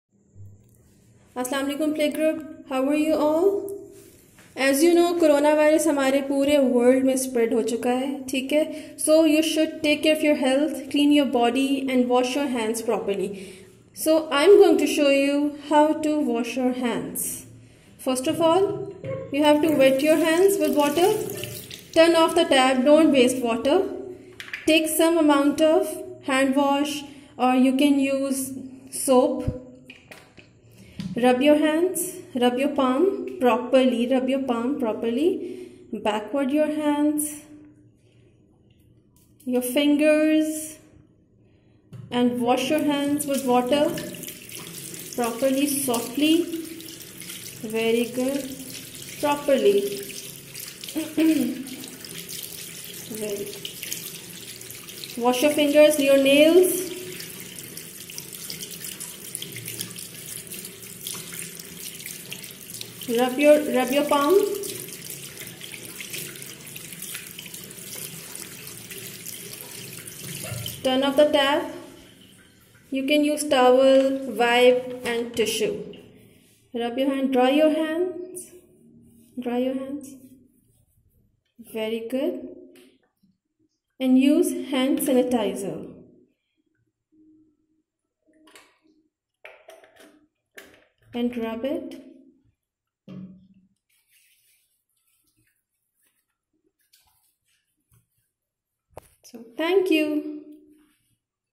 Hand Washing
washing hands is an essential part of our lives. encourage your kids to wash hand before and after every meal.